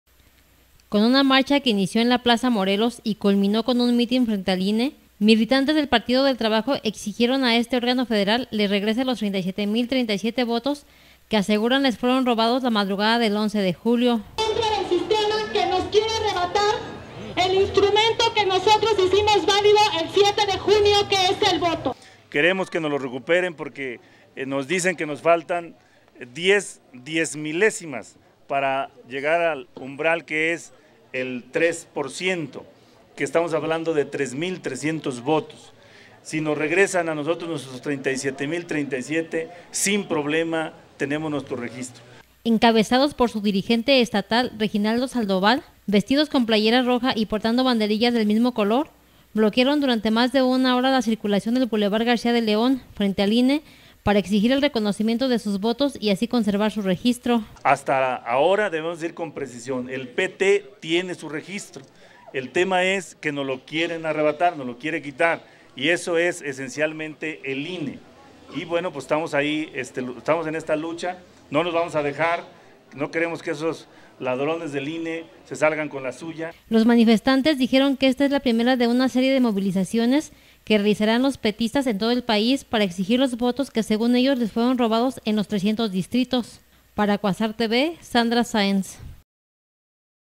Con marcha exige PT al INE le recupere más de 37 mil votos
Con una marcha que inició en la plaza Morelos y culminó con un mitin frente al INE, militantes del Partido del Trabajo, exigieron a este órgano federal les regrese los 37 mil 037 votos que aseguran les fueron robados la madrugada del once de julio…